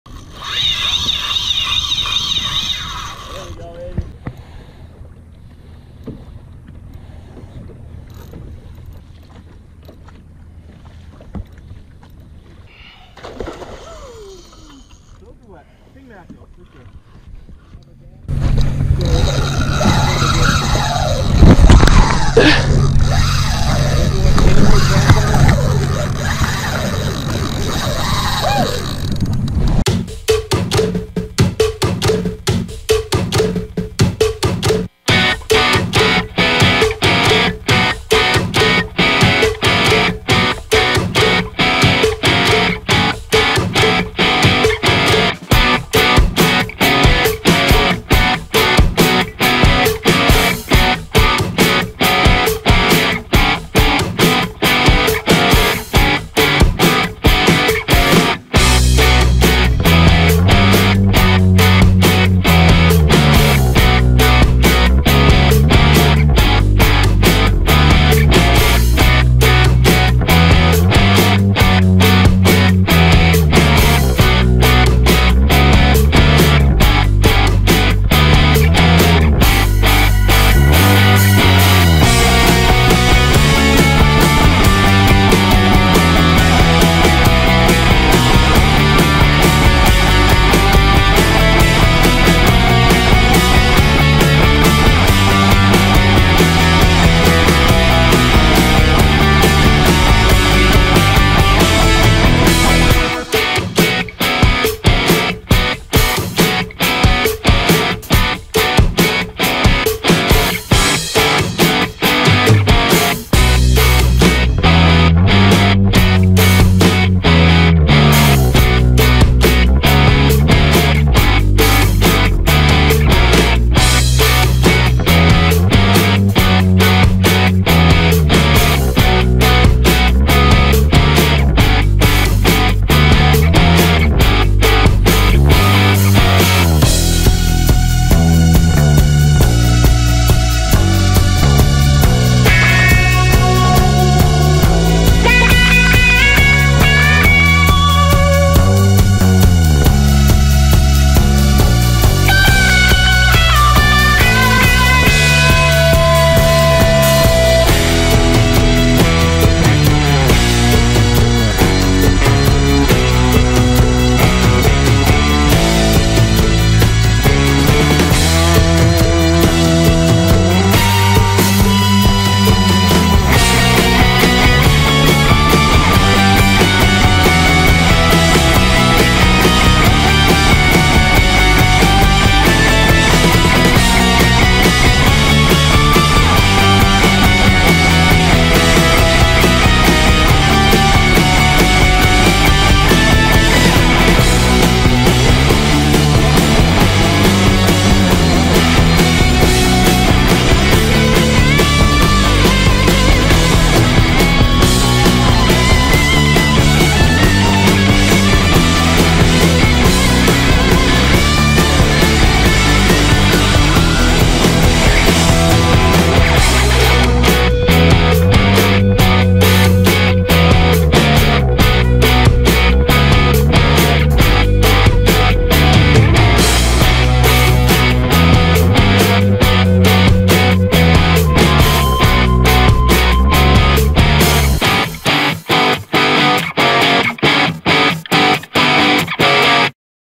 Unforgettable kayak fishing moments 2018. Big fish from a small boat.
Catching a premium game fish from a kayak.